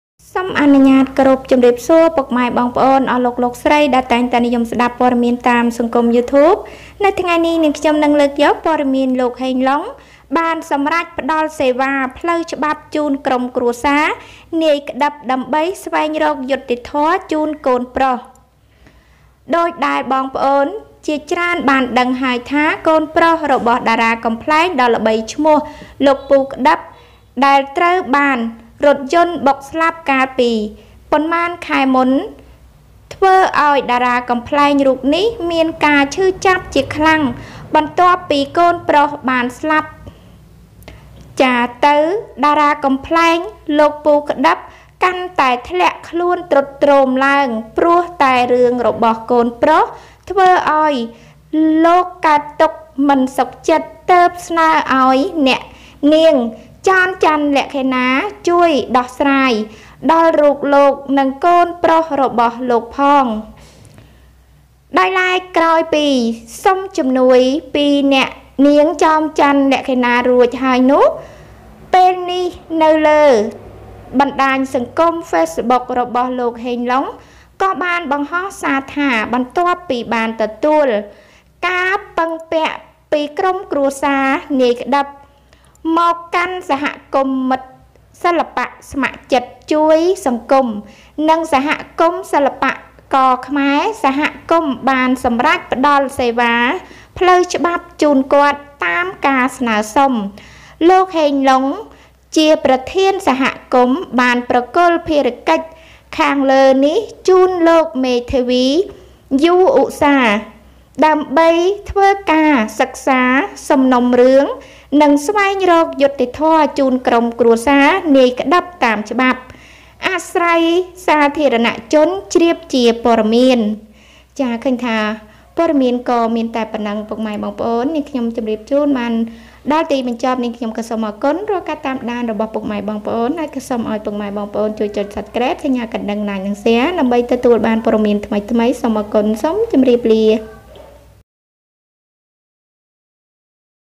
លោកហេង ឡុង បានសម្រេចផ្ដល់សេវាផ្លូវច្បាប់ជូនក្រុមគ្រួសារ នាយ ក្ដឹប ដើម្បីស្វែងរកយុត្តិធម៏ជូនកូនប្រុស
Heng Long has decided to provide legal services to the clerk's family to seek justice for his son

channel/KhmerNewsToday